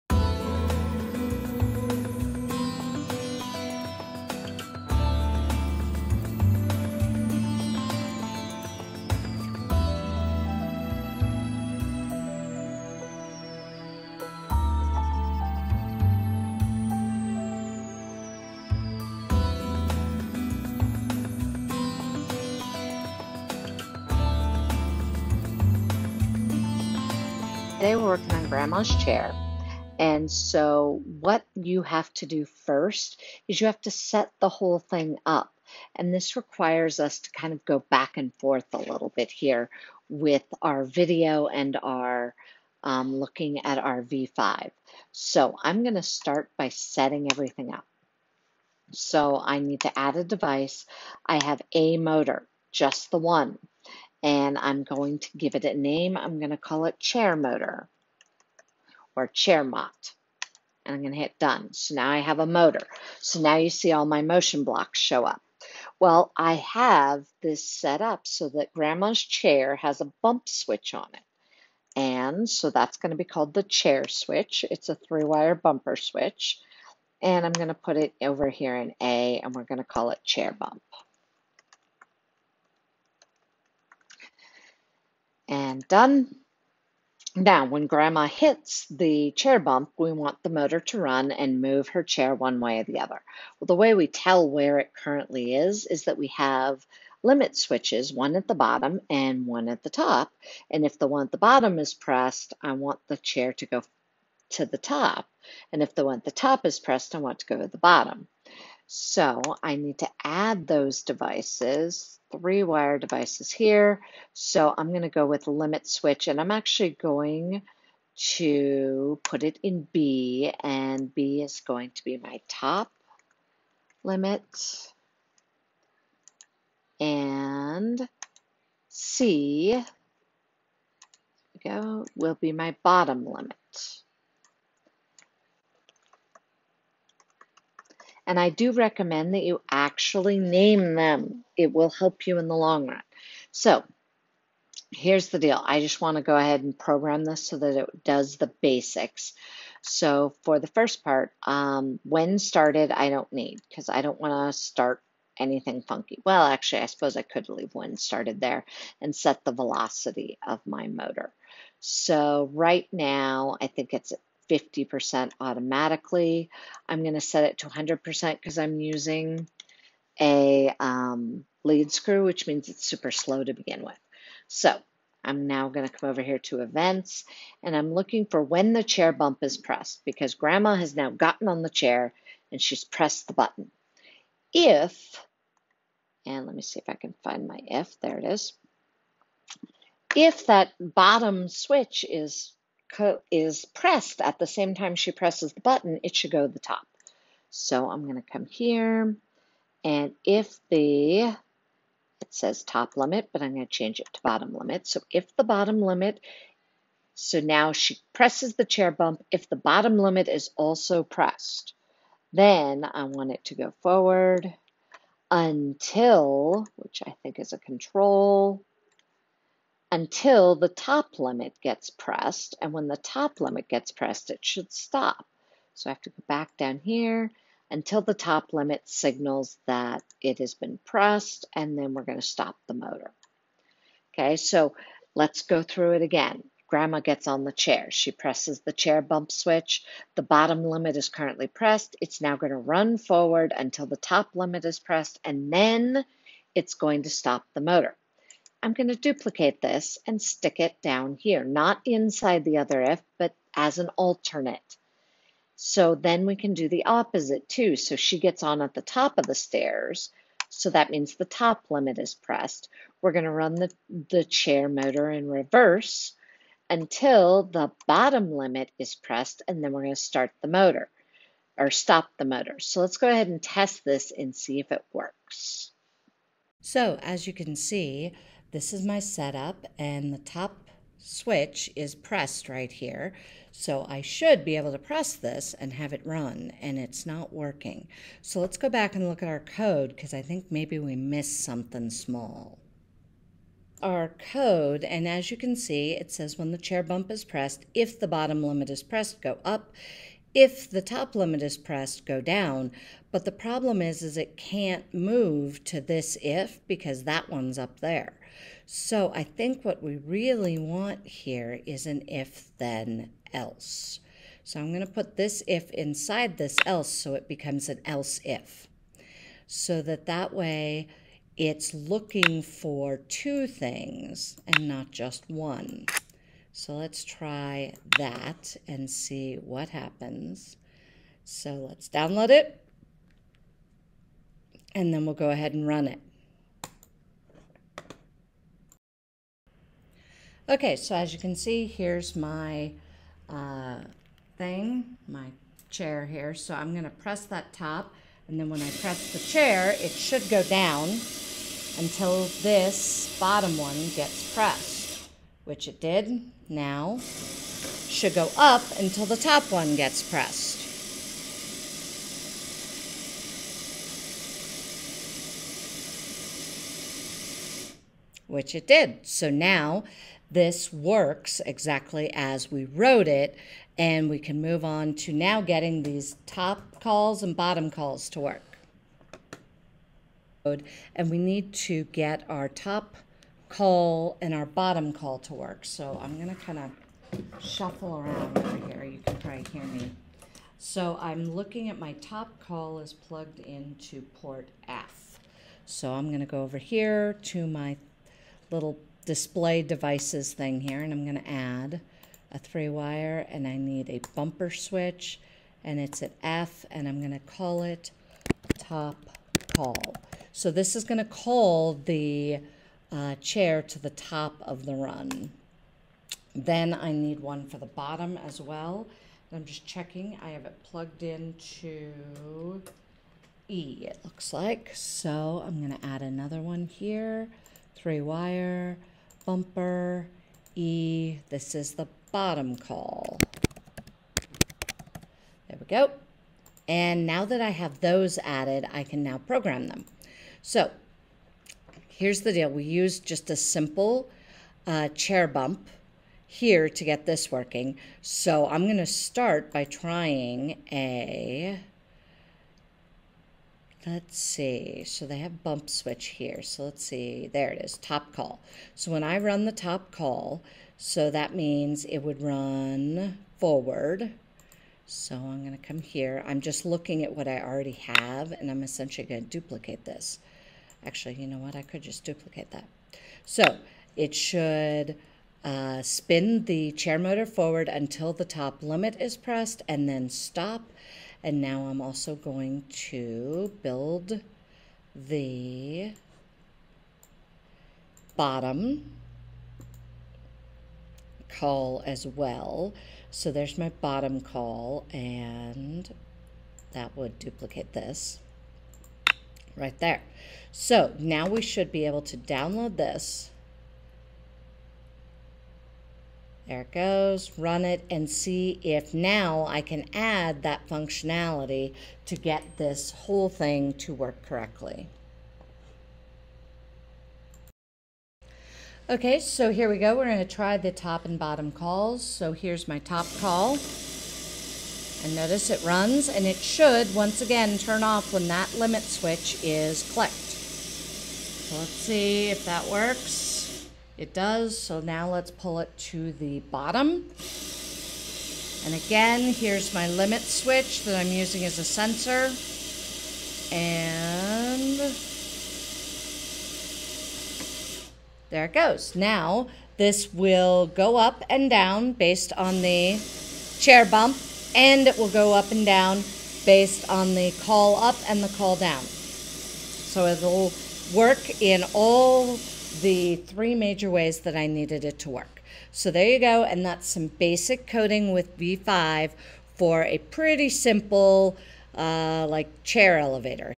V5 Grandma's Chair programming
Using V5 to set up and program a stair elevator with limit switches as digital sensors.